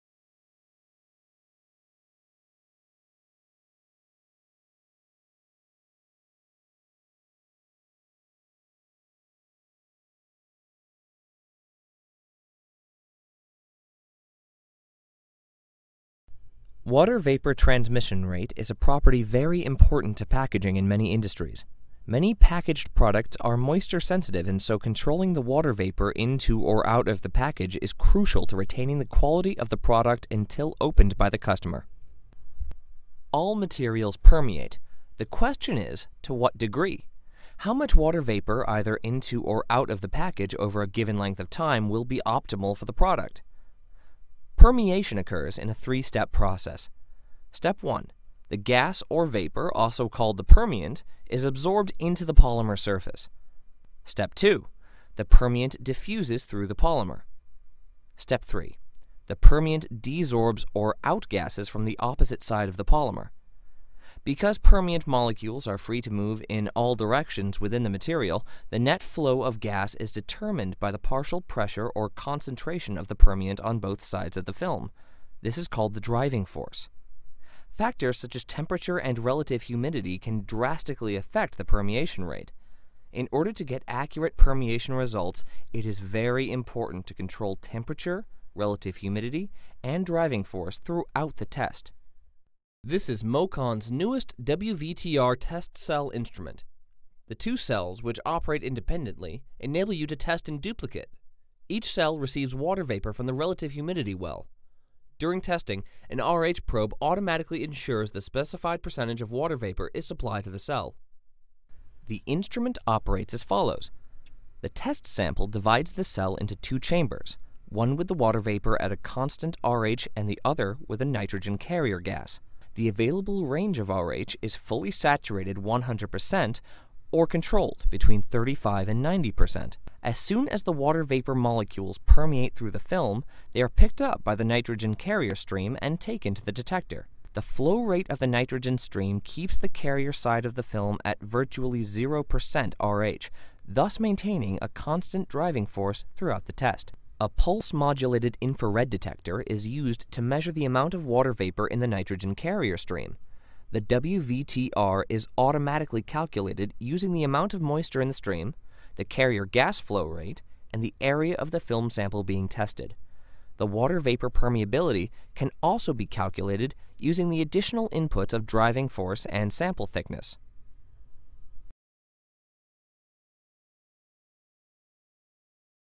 Understanding WVTR Water Vapor Transmission Rate Testing | AMETEK MOCON
Permeation is the movement of a gas or vapor through a semi-permeable barrier such as the wall of a film, whole package, medical device, or even a flexible electronic display. The gas or vapor is driven to move from an area of high concentration to an area of low concentration. This video shows a simplified explanation of how water vapor transmission rate tests are conducted.

To ensure packaged products or packaging materials will provide the desired product shelf life, protection and safety – it is critical to understand the gas permeation rates through both the barrier materials and the final finished package, and select packaging materials and designs that can meet your shelf life needs.

Introduction: 0:00
Basics of Permeation: 0:33
WVTR Testing With OX-TRAN 2/22: 1:32
Side View Diagram of Test Cell: 1:52
Closing: 2:50